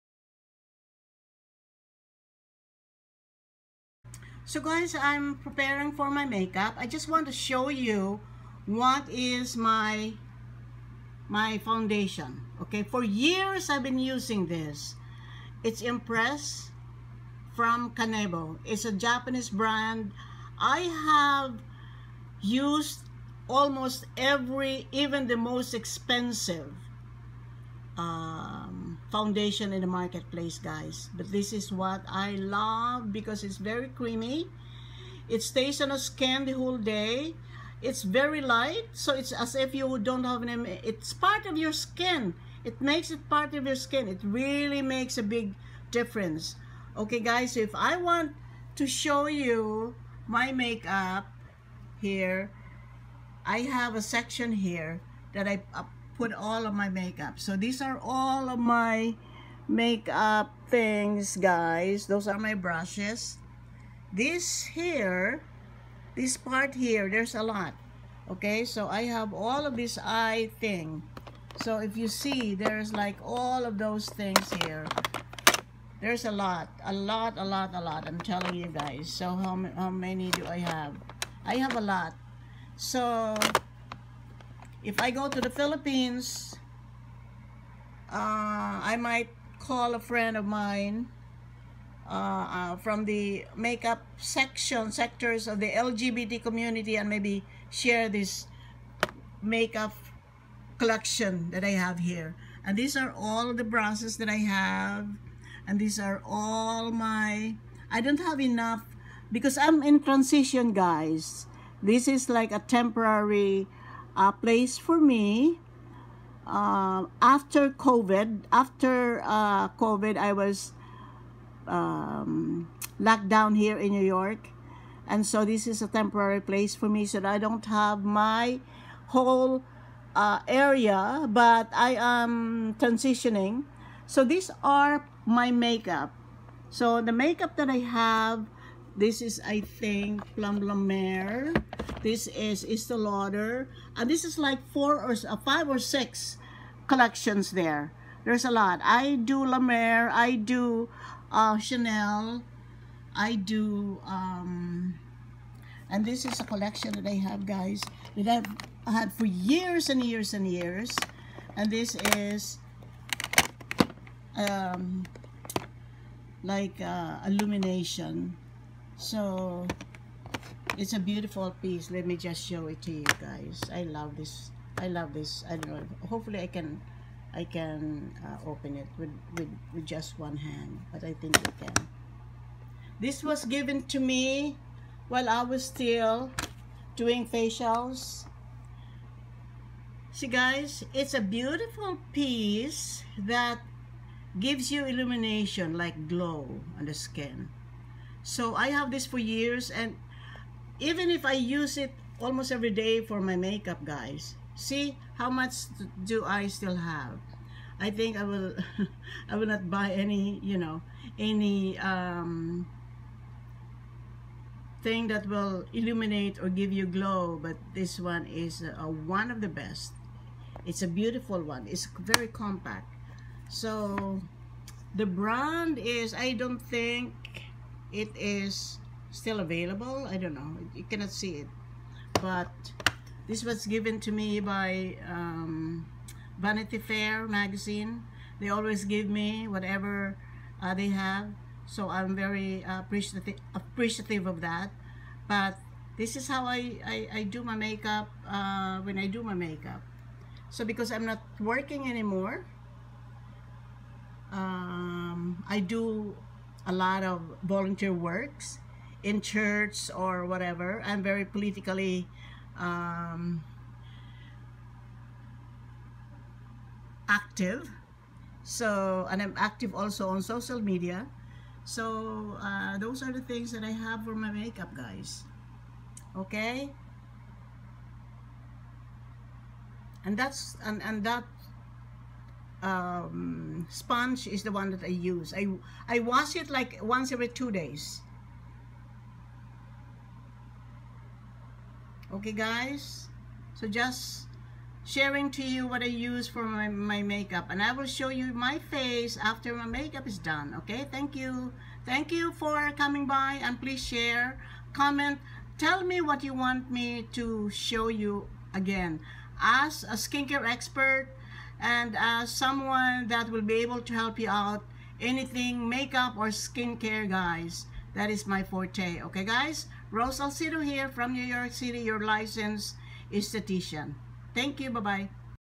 How to apply make up from drab to fab. An easy, 10 minute makeup application.
Hello my friends in the beauty world.  We are back and with a bang.  Come enjoy and join us in this transforming makeup, easy and fast.  I can do my make up in less than 10 minutes.  I am just showing you the "after" make up. The before can be shown in a separate video, please watch both.

My Products:
SPF30 Advanced Protection SPF30
Soap and Glow (Soap for facial skin)
Soap for Psoriasis and Exzema